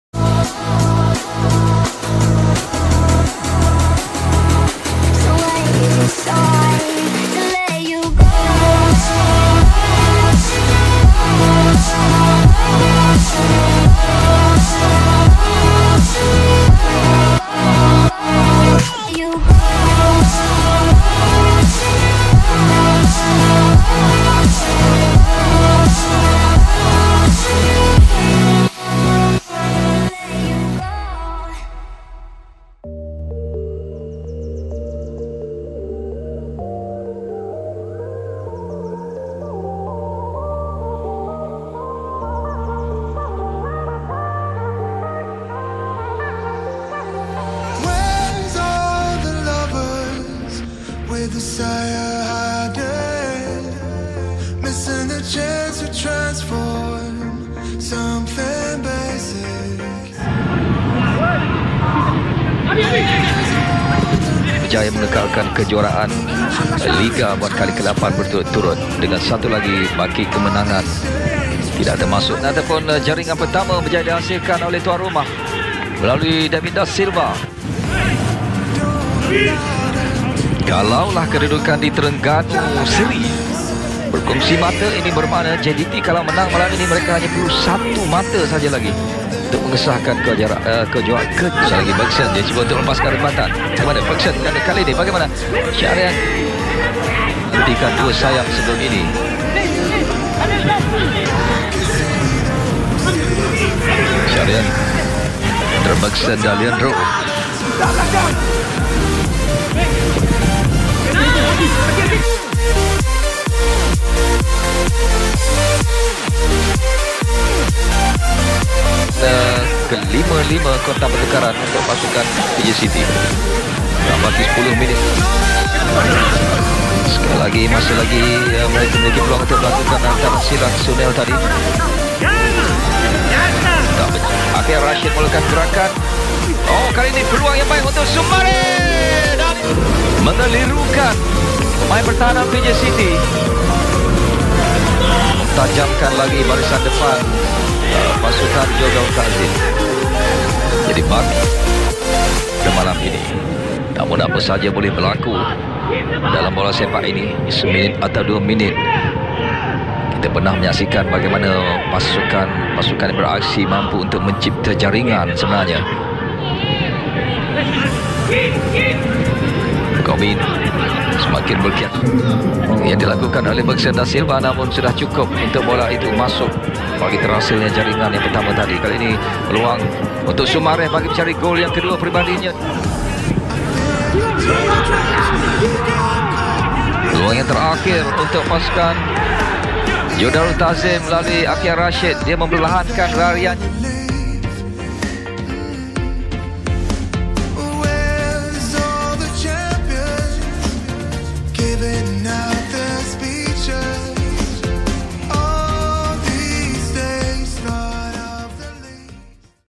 SYAHRIAN ABIMANYU - Full Skills, Intercept & Passes vs PJ City 2021
Tampil dari Bangku Cadangan, Syahrian Abimanyu Bantu JDT Kalahkan PJ City 0-2

Dukung terus channel ini agar kami terus semangat dalam memberikan konten-konten menarik dari dunia sepakbola dalam dan luar negeri.
Terima Kasih.